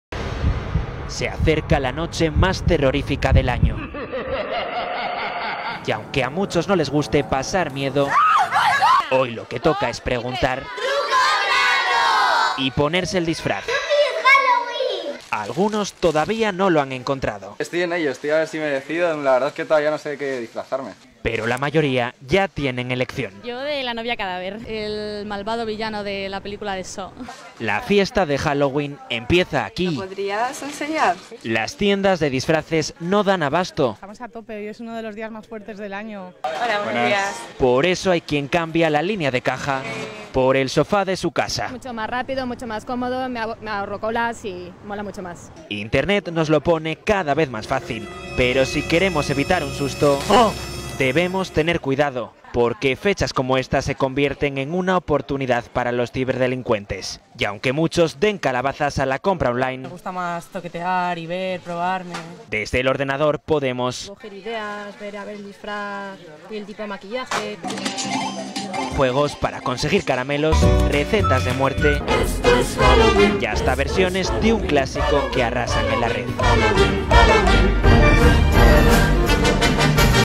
Llega Halloween, la fiesta más terrorífica del año
Es Halloween, una tradición que llega desde EEUU sigue ganando fieles en nuestro país. Es 31 de octubre, día de disfraces y de ir casa por casa con el famoso 'truco o trato' para conseguir golosinas.